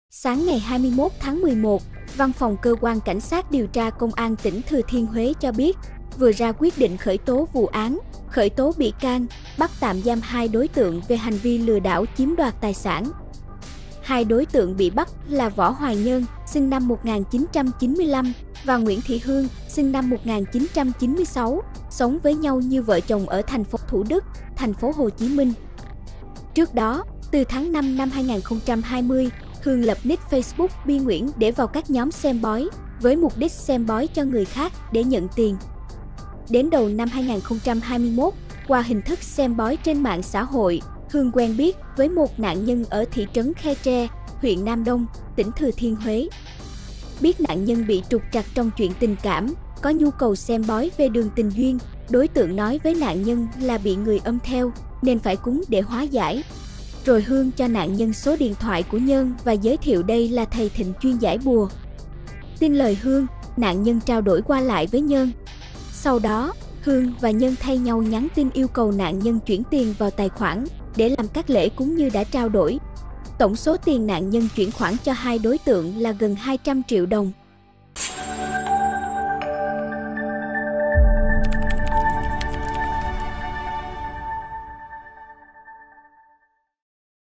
Xem bói trên mạng xã hội để lừa đảo, 2 đối tượng bị khởi tố
Sáng ngày 21-11, Văn phòng cơ quan Cảnh sát điều tra Công an tỉnh Thừa Thiên Huế cho biết, vừa ra quyết định khởi tố vụ án, khởi tố bị can, bắt tạm giam 2 đối tượng về hành vi "Lừa đảo chiếm đoạt tài sản".